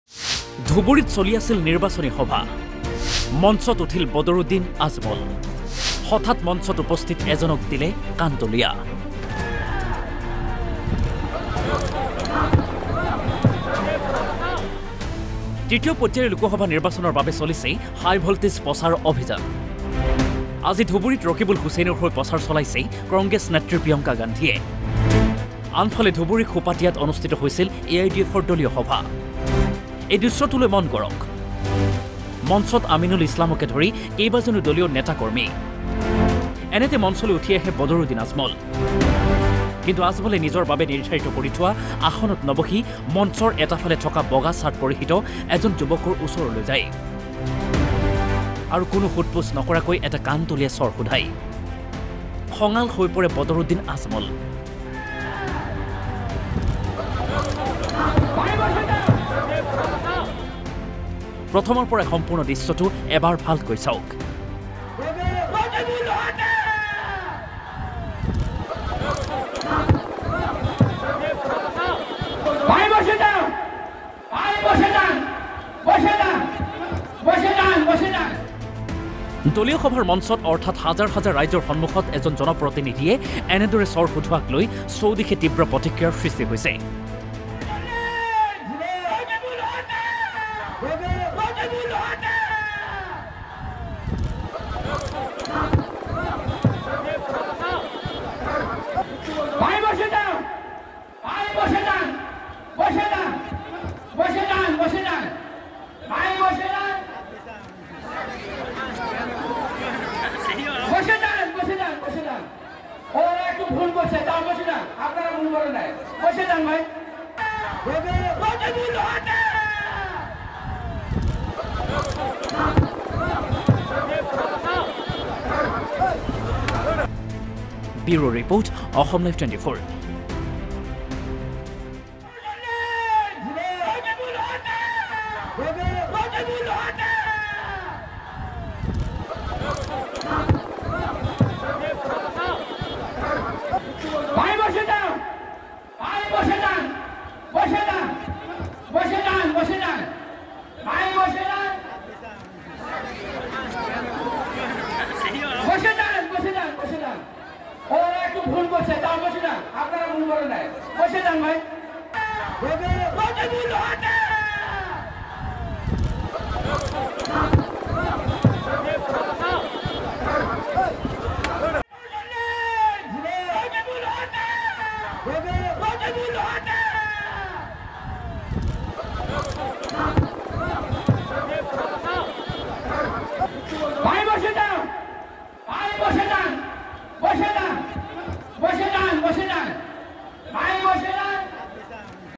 মঞ্চতে বদৰুদ্দিন আজমলে এজনক দিলে কাণতলীয়া ৷
মঞ্চতে বদৰুদ্দিন আজমলে এজনক দিলে কাণতলীয়া ৷ নিৰ্বাচনী সভাত হাজাৰ হাজাৰ ৰাইজৰ সন্মুখত আজমলে কাণতলীয়া শোধোৱা কাৰ্যত ব্যাপক প্ৰতিক্ৰিয়া৷